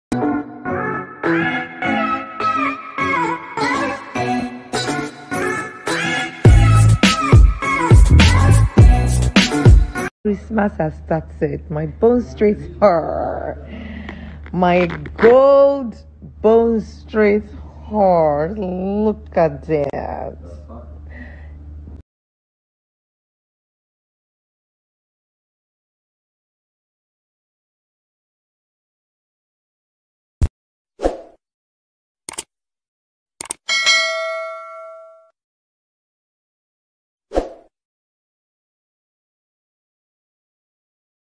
Toke Makinwa Shows Off N1.2 Million Worth "Bone Straight" Hair A Friend Gifts Her For Christmas
Toke Makinwa Shows Off "Bone Straight" Hair That Worths Over N1.2 Million As Christmas Gift